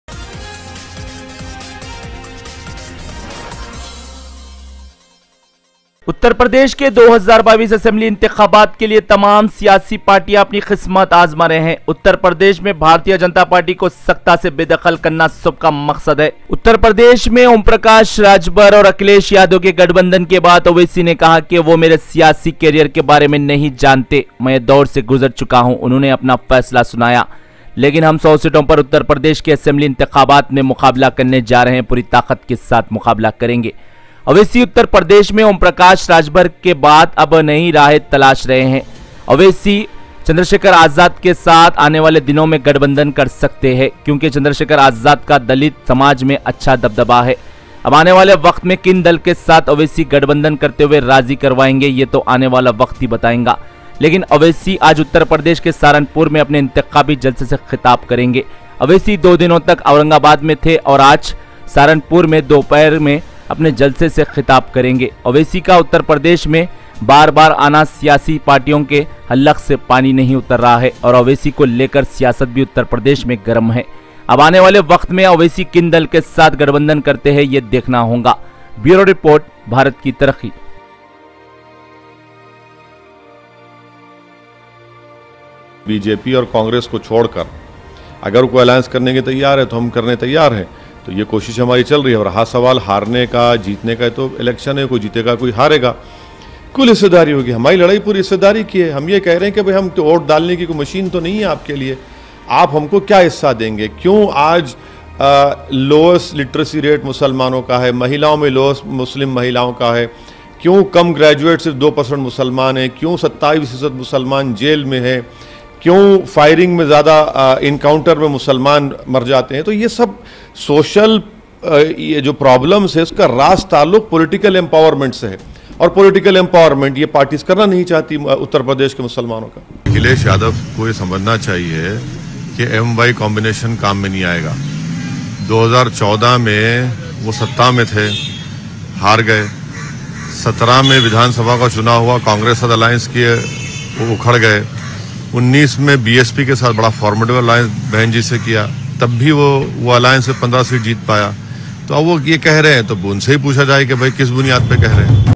Owaisi may likely make alliance with Chandrashekhar Azad & can do Jalsa in Saharanpur,UP | Today
Owaisi may likely make alliance with Chandrashekhar Azad & can do Jalsa in Saharanpur,UP | Today | BT NEWS